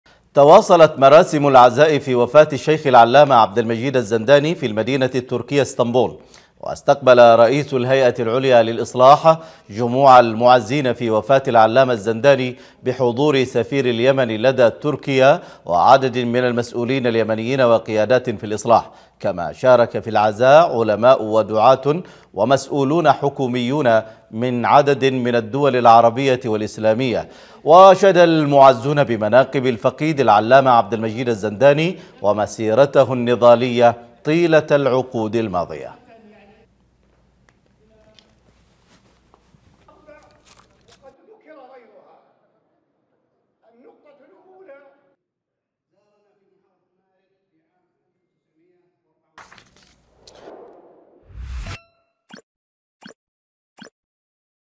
تواصل مراسم العزاء في وفاة الشيخ الزنداني ورئيس الهيئة العليا للإصلاح يستقبل جموع المعزين في اسطنبول
قناة_سهيل# ‪